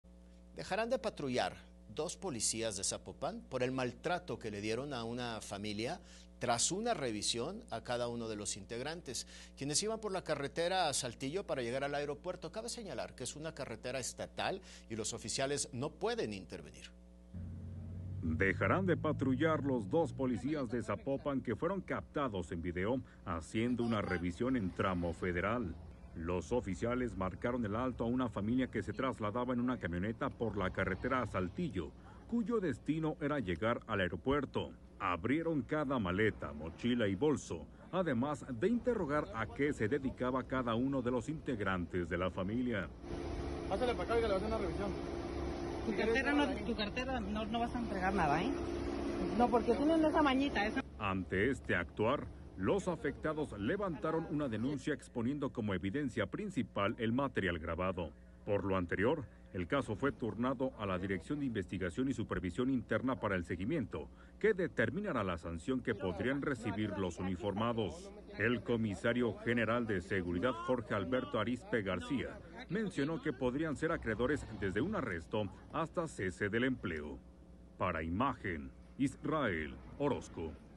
Suspenden a dos Policías de Zapopan por Agresiva Revisión | Noticias GDL con Rey Suárez
Dos policías de Zapopan fueron suspendidos de patrullar tras ser captados realizando una revisión agresiva a una familia.